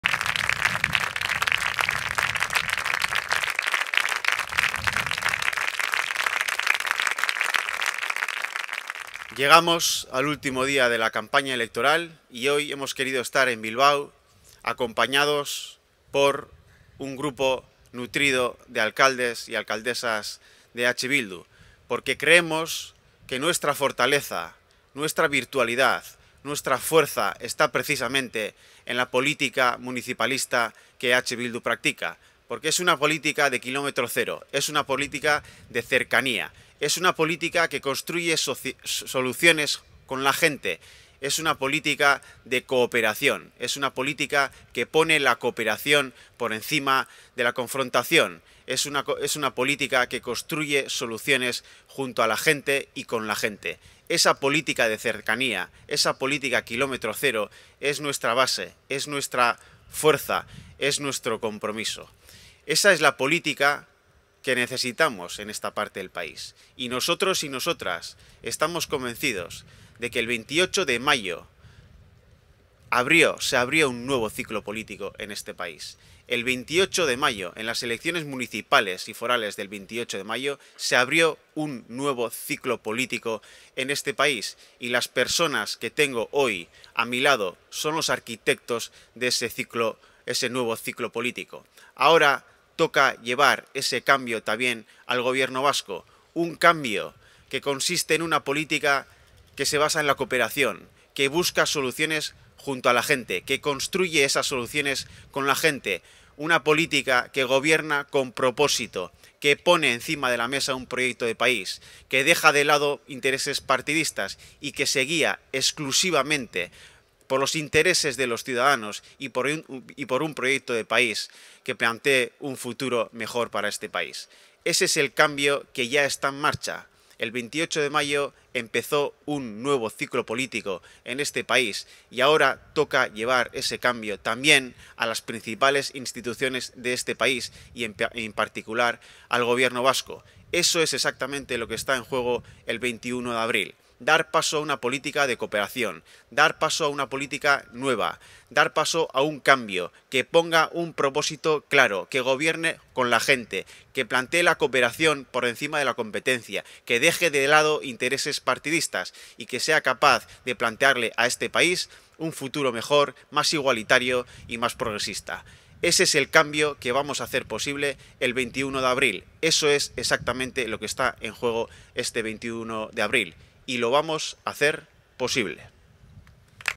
Pello Otxandiano recibe en Bilbo el respaldo de decenas de alcaldes y alcaldesas de EH Bildu
El candidato de EH Bildu a lehendakari, Pello Otxandiano, ha participado esta mañana en un acto político en Bilbao junto con decenas de alcaldes y alcaldesas de la coalición que le han transmitido su apoyo de cara a las elecciones del domingo. Otxandiano ha ensalzado la labor de EH Bildu al frente de tantos ayuntamientos y ha manifestado su voluntad de llevar al Gobierno vasco ese liderazgo y esa prueba de que hay otras maneras de hacer política.

En este sentido, ha destacado que «los alcaldes y las alcaldesas son los verdaderos arquitectos del Cambio Político, construyendo comunidades más fuertes, convirtiendo visiones en realidades tangibles e impulsando transformaciones positivas en la vida de sus ciudadanos. El liderazgo que demuestran nuestros artesanos urbanos es lo que queremos llevar al Gobierno vasco».